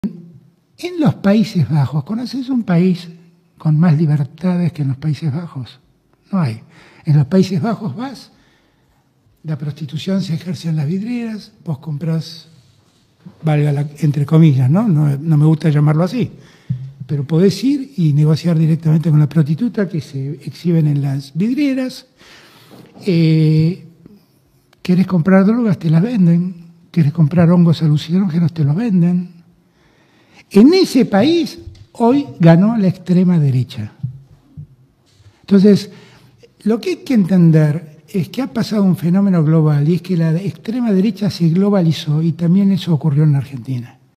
Alberto ¿Qué fumó?: Surgimiento de la Ultraderecha, prostitución y drogas en Países Bajos
El 27 de mayo de 2024, Alberto Fernández en su delirio relaciona la prostitución, las drogas en Países Bajos con el surgimiento de la ultraderecha en este país... ¿Qué tendrá que ver? No hay relación alguna. Este sujeto definitivamente no está bien ¿Qué habrá fumado?